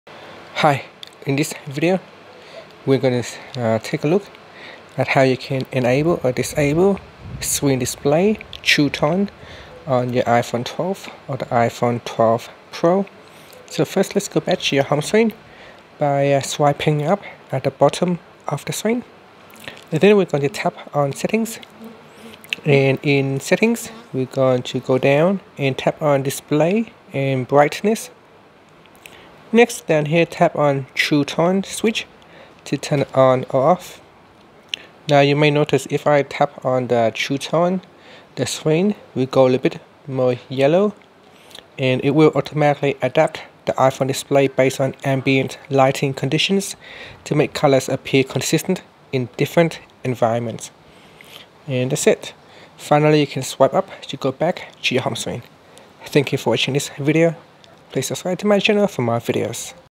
iPhone 12/12 Pro: How to Enable/Disable Screen Display True Tone
Learn how you can enable or disable screen display true tone on the iPhone 12 / iPhone 12 Pro.